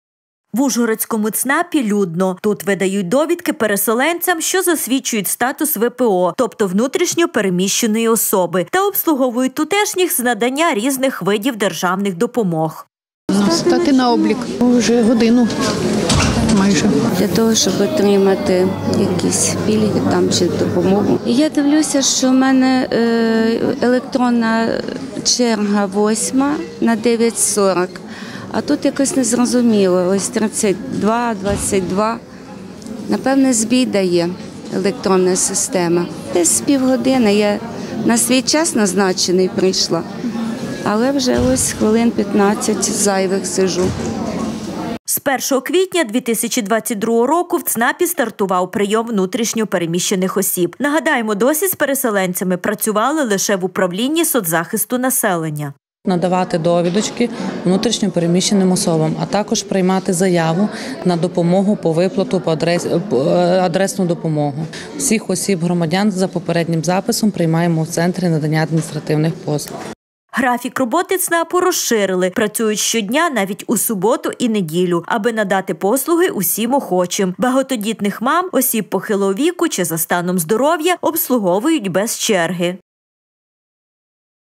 Видача довідок ВПО. Надання послуг
В ужгородському ЦНАПі – людно! Тут видають довідки переселенцям, що засвідчують статус ВПО – тобто внутрішньо-переміщеної особи та обслуговують тутешніх з надання різних видів державних допомог.З 1 квітня 2022 року в ЦНАП і стартував прийом внутрішньо переміщених осіб. Нагадаємо, досі з переселенцями працювали лише в управлінні соцзахисту населення.Графік роботи ЦНАПу розширили! Працюють щодня, навіть у суботу і неділю, аби надати послуги усім охочим. Багатодітних мам,  осіб похилого віку  чи за станом здоров’я – обслуговують без черги.